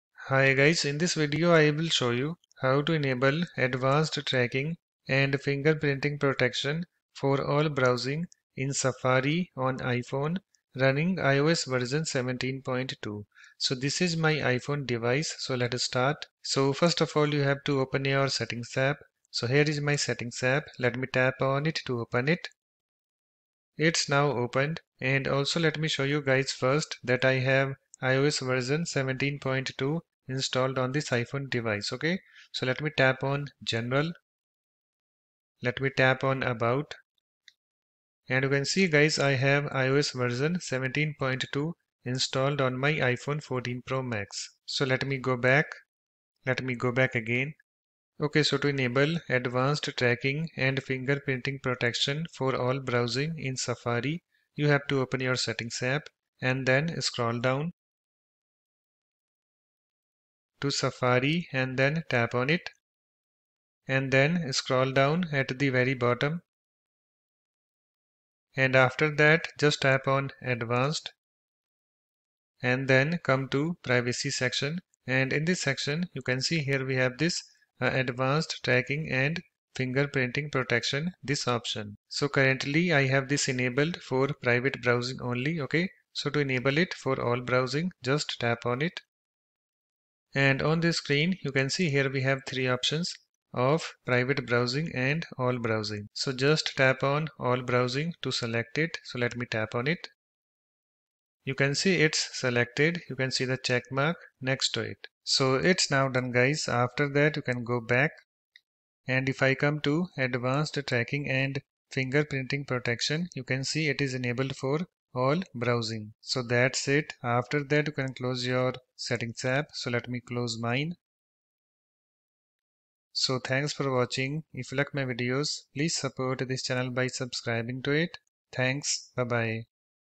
Enable Advanced Tracking & Fingerprinting Protection for All Browsing in Safari on iPhone - iOS 17.2
Hi guys, In this Video, I will show you "How to Enable Advanced Tracking & Fingerprinting Protection for All Browsing in Safari on iPhone (iOS 17.2)." Please Like and Subscribe to my channel and press the bell icon to get notifications of new videos.